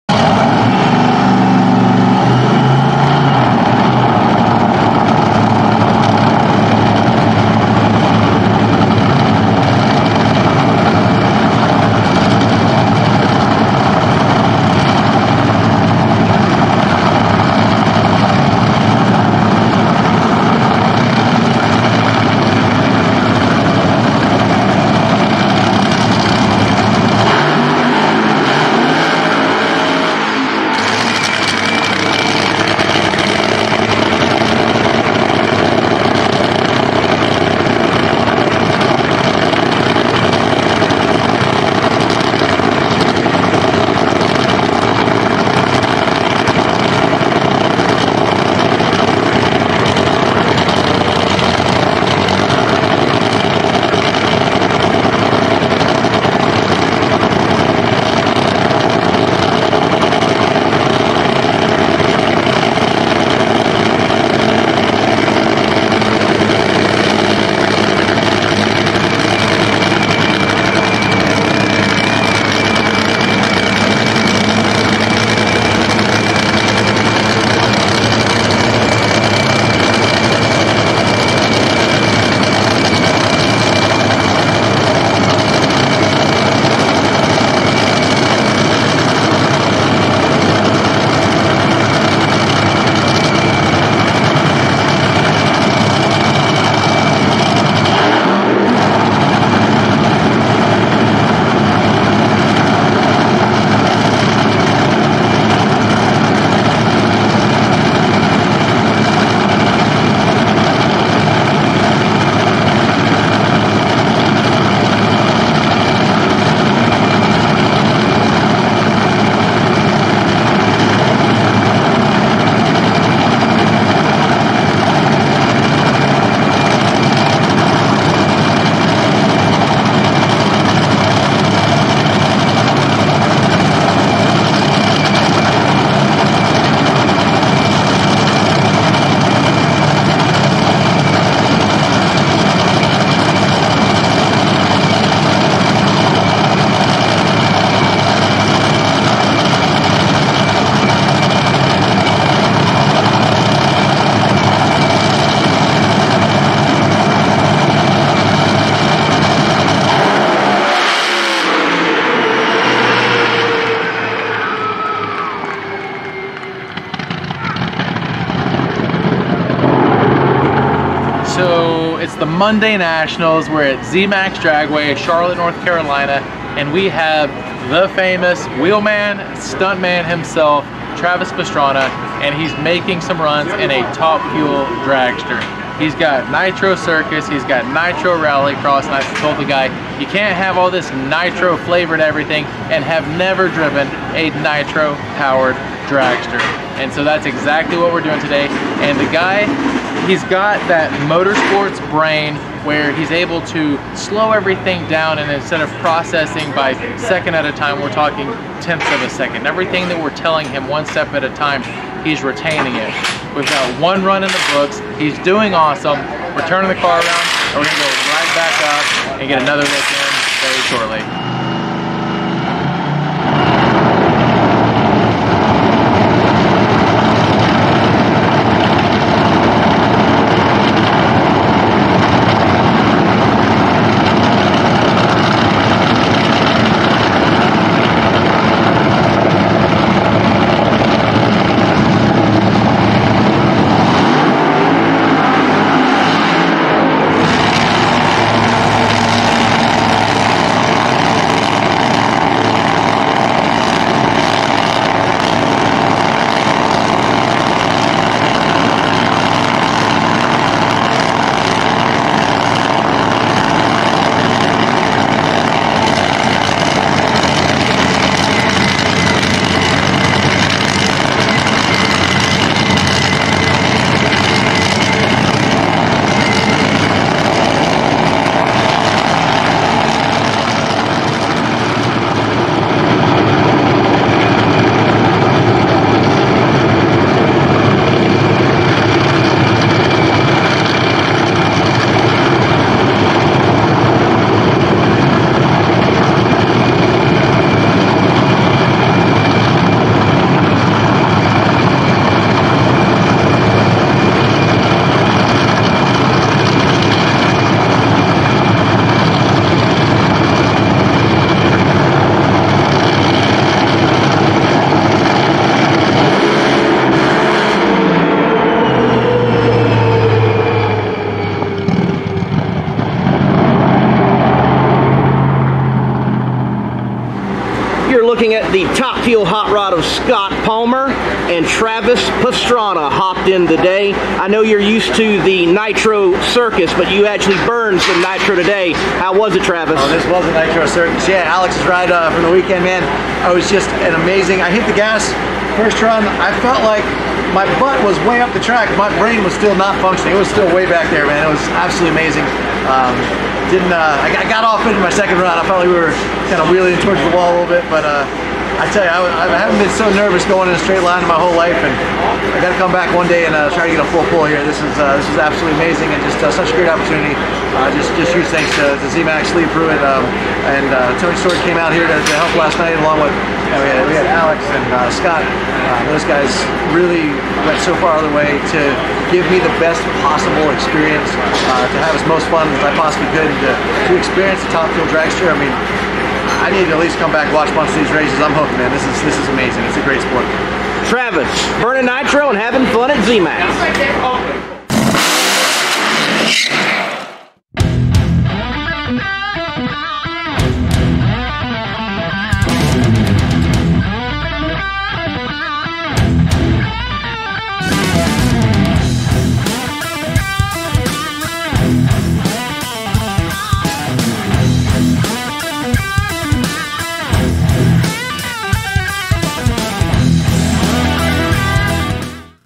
Travis Pastrana In A NHRA Top Fuel Dragster At zMAX Dragway | Scott Palmer Racing
Nitro worlds collided today at zMAX DRAGWAY! The world known man of motorsport action Travis Pastrana made hits today in a 10,00+ horsepower NHRA top fuel dragster. This is such a cool day for drag racing, Travis, and everyone involved. Great job by Alex Laughlin to bring parties together. Thanks to the showman of drag racing himself Scott Palmer lending his Scott Palmer Racing dragster to make it all possible along with a team that knows how to get the job done. There were others involved as well such as Aaron Brooks, Leal Pruett, Tony Stewart, and even Lyle Barnett Racing was hanging out. Travis, good job today and we can't wait to see you back behind the wheel. LOOK FORWARD to all the behind the scences action from React104, Alex Laughlin, Scott Palmer, and more.